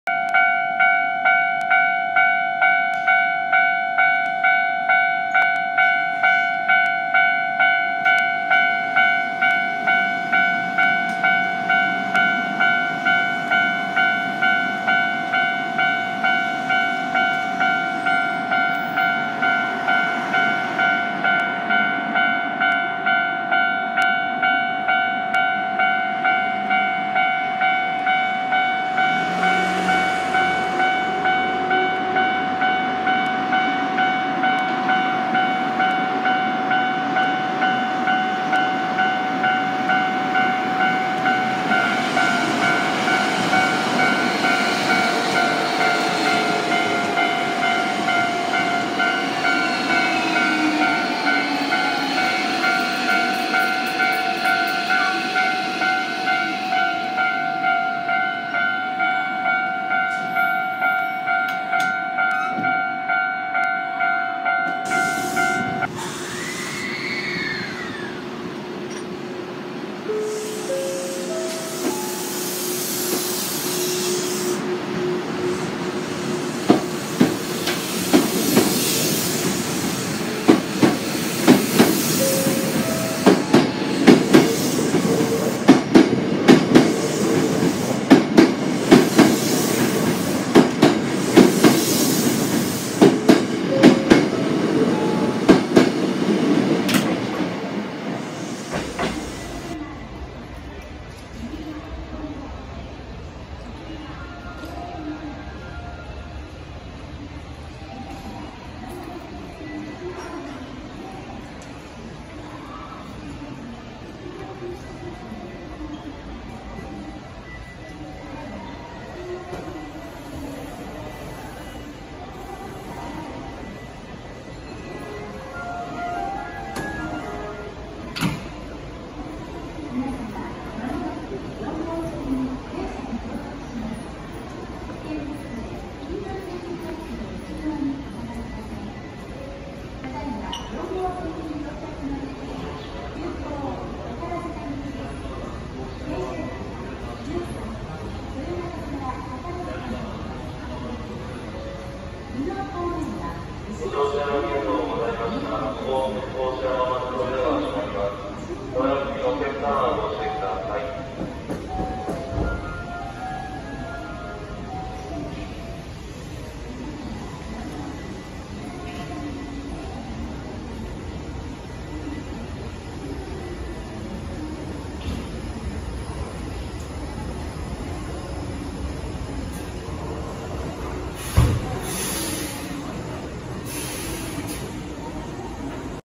阪急宝塚線1000系を見る
阪急宝塚線1000系1015F普通雲雀花屋敷行き1018F普通大阪梅田行き1001F普通雲雀花屋敷行き6000系6015F急行宝塚行き